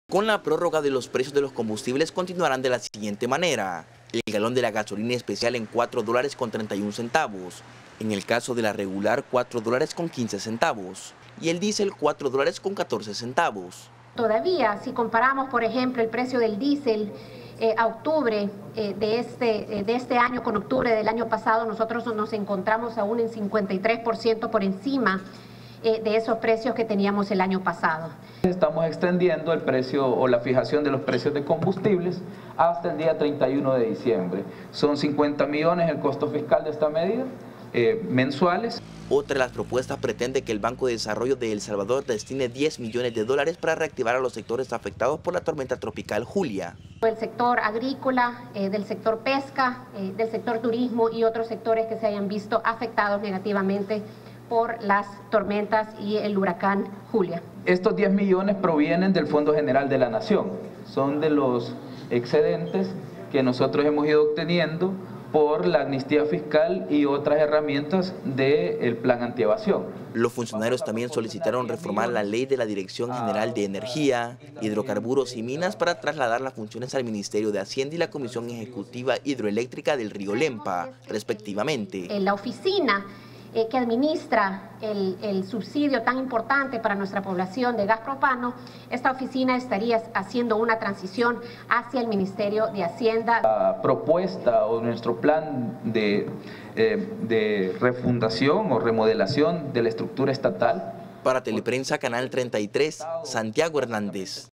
Teleprensa33 | El Ministerio de Economía presentó una prórroga para los precios de los combustibles
La ministra de Economía, María Luisa Hayem presentó este martes a la Asamblea Legislativa un paquete de iniciativas de ley entre las que incluye prorrogar la fijación de los precios de los combustibles.